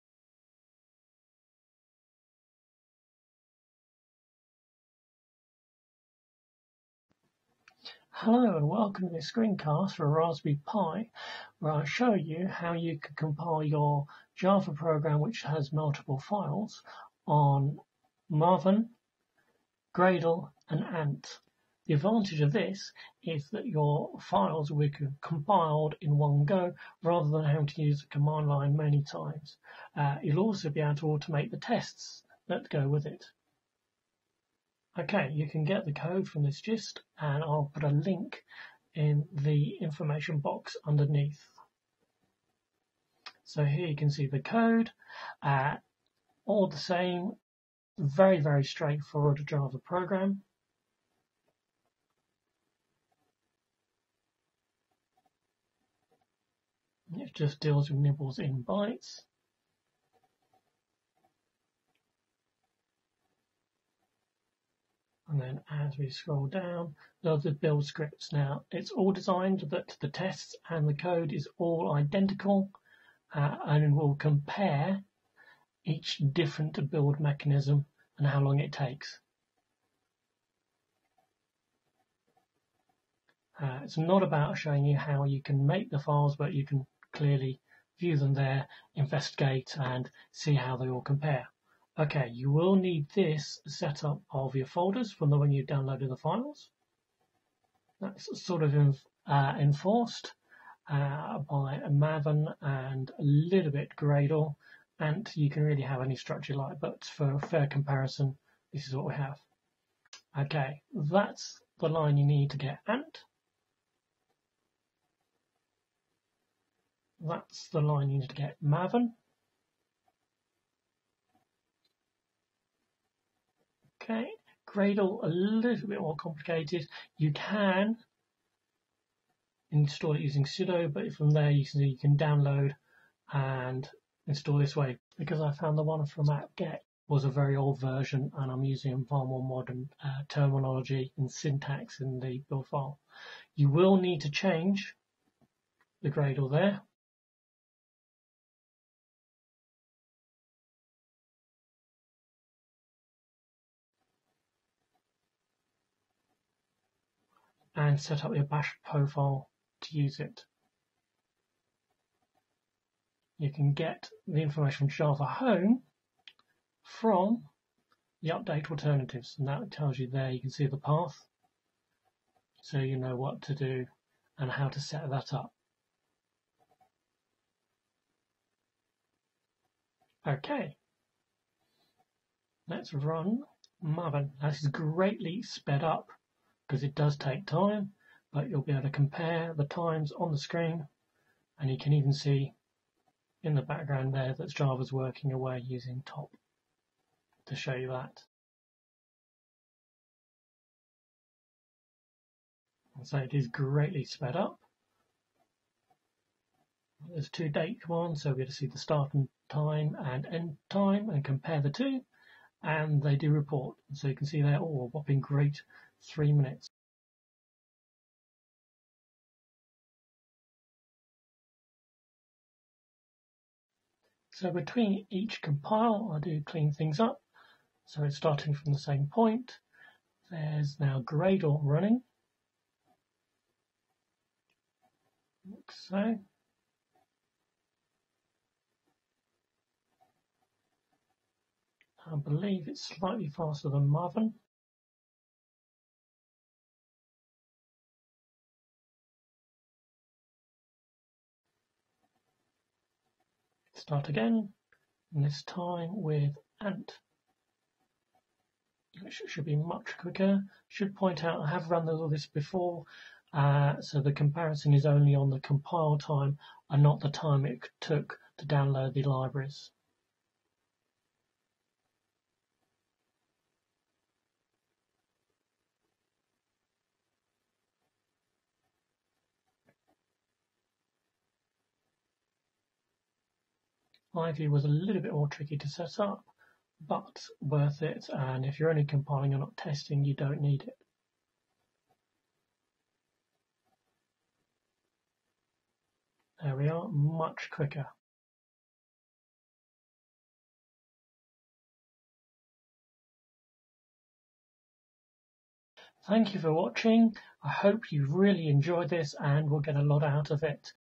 Maven, Gradle and Ant for Java on the Raspberry Pi B+
Introduction:

Compiling more than one Java file and assembling into a run-able Jar can be time consuming and repetitive when you use the command line.  In this screen cast I demonstrate and compare three build tools that allow you to automate this process.  In addition each tool builds and runs the associated tests for extra confidence in the code.

The code used can be found on GitHub's Gist - link below - there are lots of guides and documentation around that help you construct the build scripts.  But I have attempted to keep the scripts used as minimal and understandable as possible.